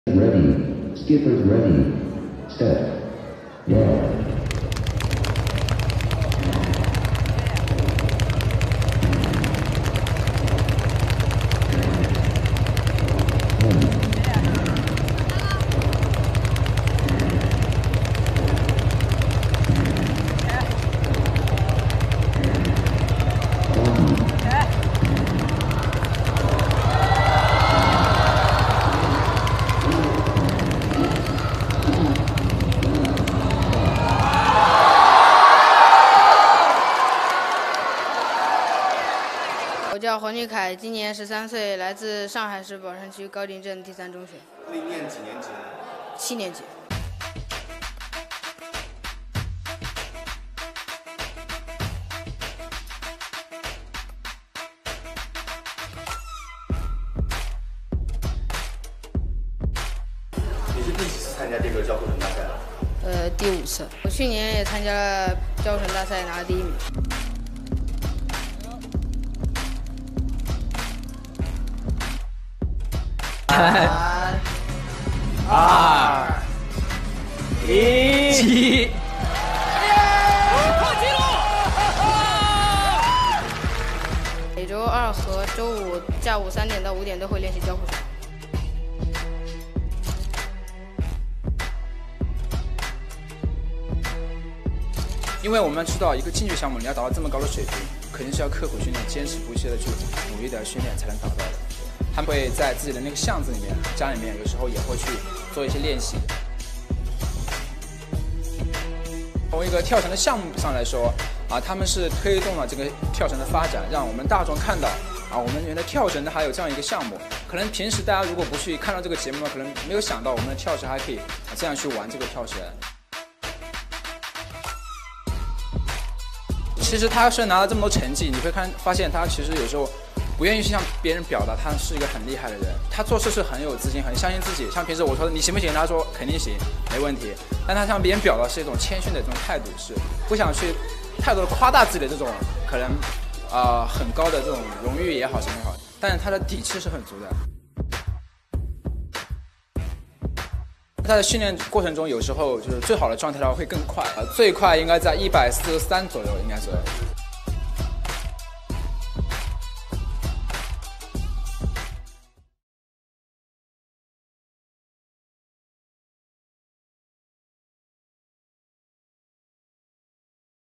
Teenager breaks double-dutch world record with INCREDIBLE SKILL
A teenager in China has stunned thousands with his impressive rope-jumping skills at a competition while breaking his own world record. Video of the performance by 13-year-old student Huang Junkai shows him completing 136 jumps in 30 seconds at the Double Dutch Contest Shanghai 2018 on Sunday. That's an average of 9.07 jumps per second. Huang, along with teammates Jiang Dali and Jin Zhenyu, had set a Guinness World Record last year with 133 jumps in 30 seconds on the CCTV program Impossible Challenge.